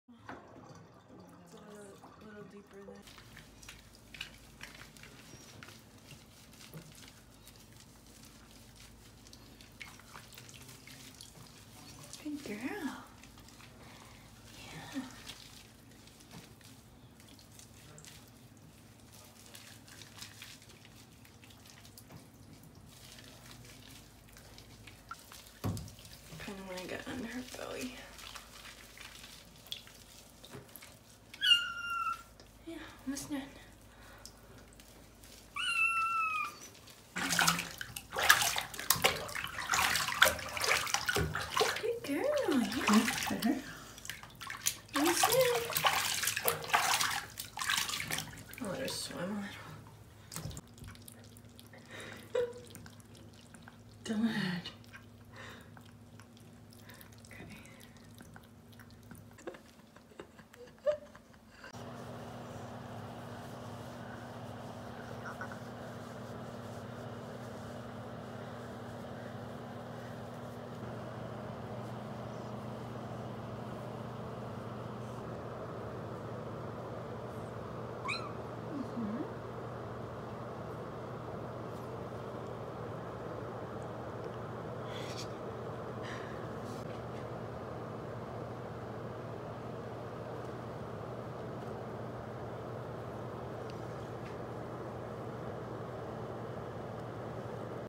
Maine Coon Kitten Molly's First Bath!
Molly's first bath was on 1/22/2018. She did great, and I think she liked it at the end because she didn't want to come out.